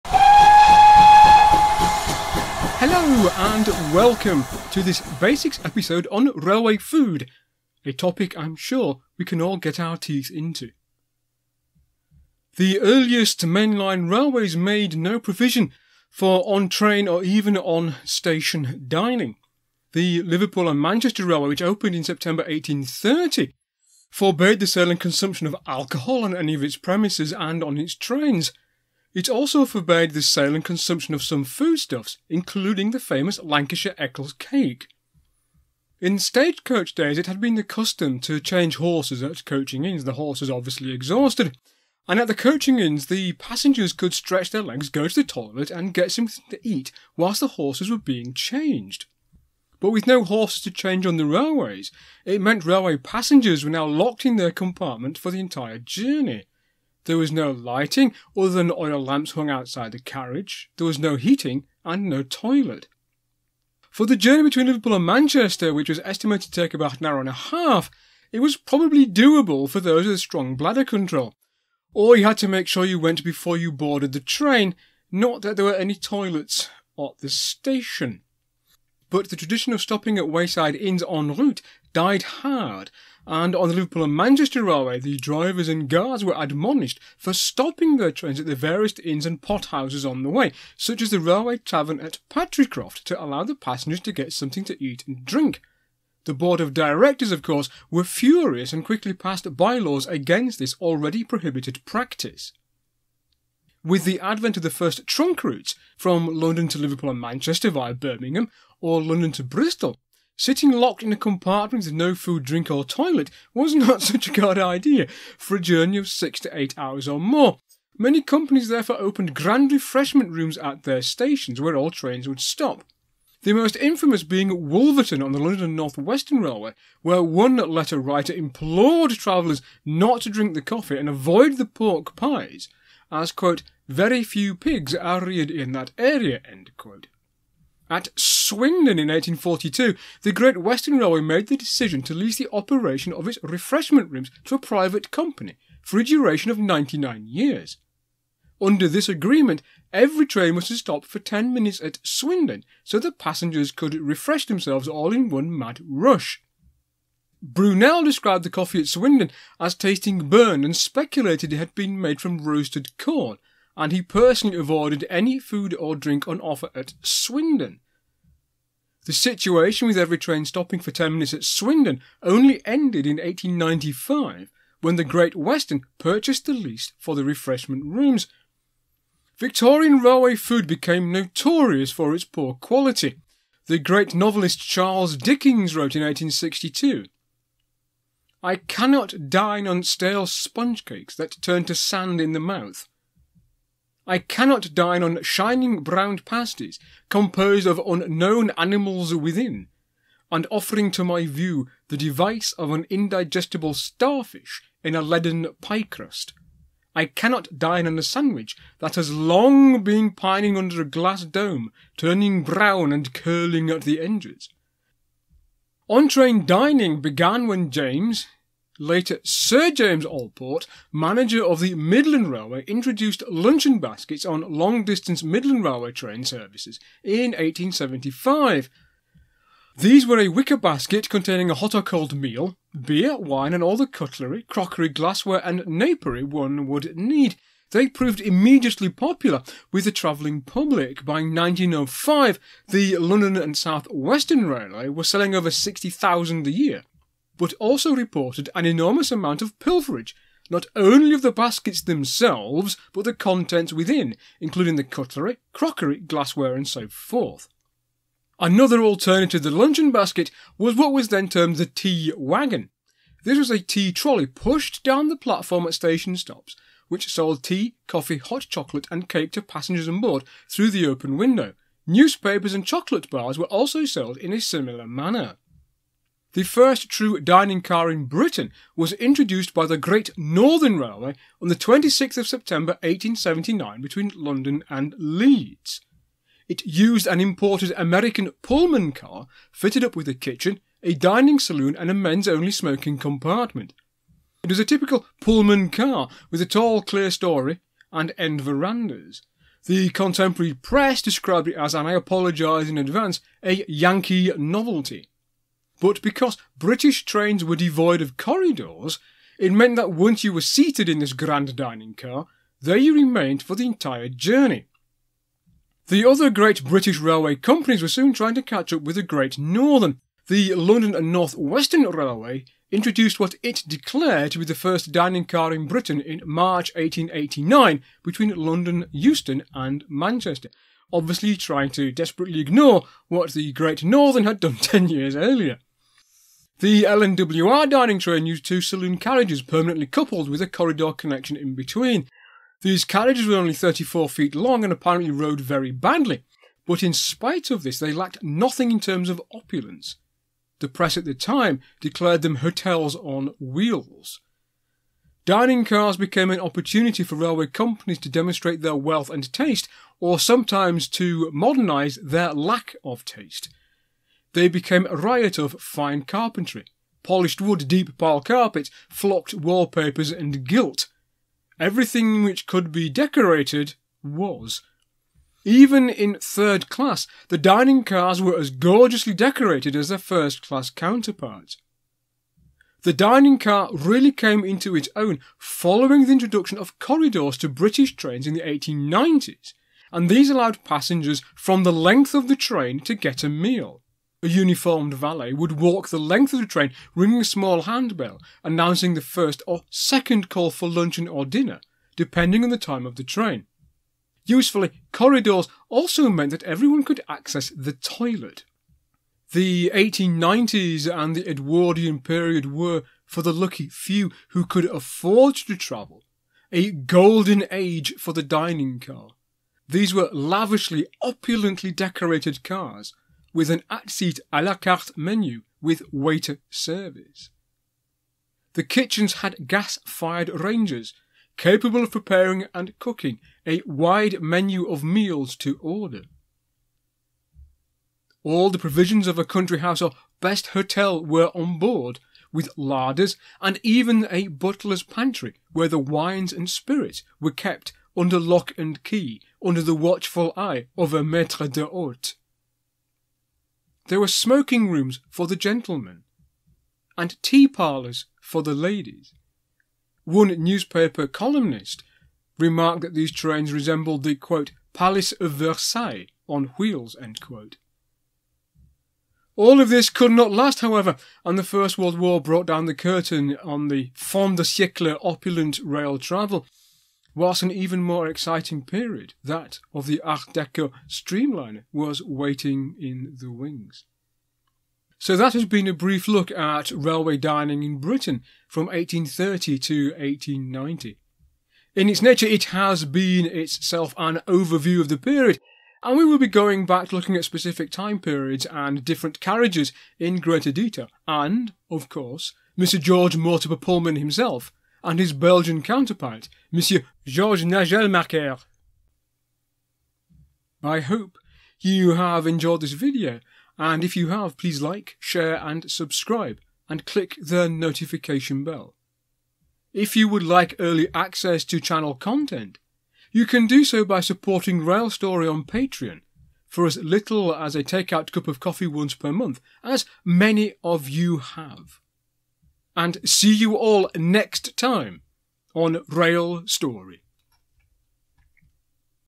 Railway Food: The Basics
Dining on the move was a new phenomenon in the nineteenth century. Stage Coaches stopped at 'Coaching Inns' en route to change horses and so passengers could refresh themselves. The new railways didn't need to change horses, but passengers still needed some refreshment. At first railway companies banned the sale and consumption of food and alcohol on their trains and premises, but later saw this was a useful revenue stream. Finally, in the 1890s the Dining Car, sumptuous palaces on wheels, were born.

If you want to find out more about Victorian Railway Travelling check out:

"Travelling on the Victorian Railway"

"The Victorian and Edwardian Railway in Old Photographs"